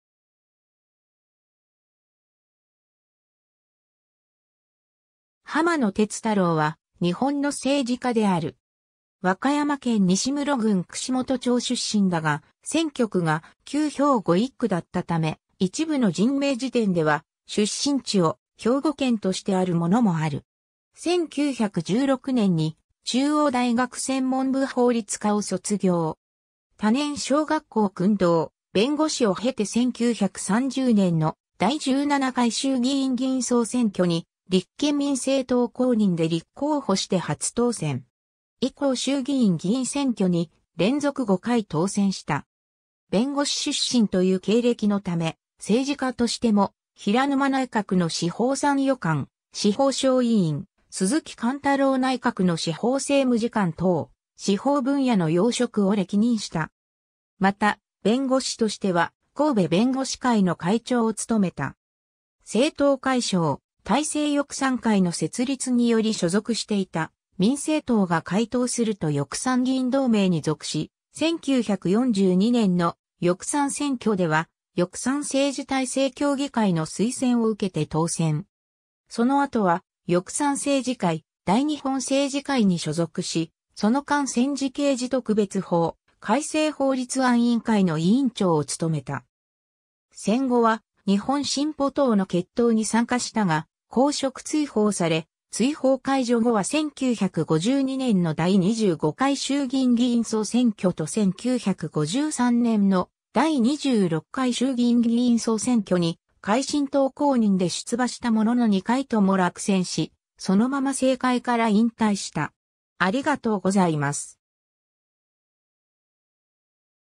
浜野徹太郎
兵庫県選出の衆議院議員_(帝国議会)
浜野 徹太郎（はまの てつたろう、1885年7月26日 - 1974年1月21日）は、日本の政治家である。
和歌山県西牟婁郡串本町（2005年4月1日に東牟婁郡古座町と合併し西牟婁郡から東牟婁郡に編入）出身だが、選挙区が旧兵庫1区だったため、一部の人名事典では出身地を兵庫県としてあるものもある。
 1916年に中央大学専門部法律科を卒業。
多年小学校訓導、弁護士を経て1930年の第17回衆議院議員総選挙に立憲民政党公認で立候補して初当選。
以降衆議院議員選挙に連続5回当選した。
 弁護士出身という経歴のため、政治家としても平沼内閣の司法参与官、司法省委員、鈴木貫太郎内閣の司法政務次官等、司法分野の要職を歴任した。
 また、弁護士としては神戸弁護士会の会長を務めた。
 政党解消、大政翼賛会の設立により所属していた民政党が解党すると翼賛議員同盟に属し、1942年の翼賛選挙では翼賛政治体制協議会の推薦を受けて当選。
その後は翼賛政治会、大日本政治会に所属し、その間戦時刑事特別法改正法律案委員会の委員長を務めた。
戦後は日本進歩党の結党に参加したが公職追放され、追放解除後は1952年の第25回衆議院議員総選挙と1953年の第26回衆議院議員総選挙に改進党公認で出馬したものの2回とも落選し、そのまま政界から引退した。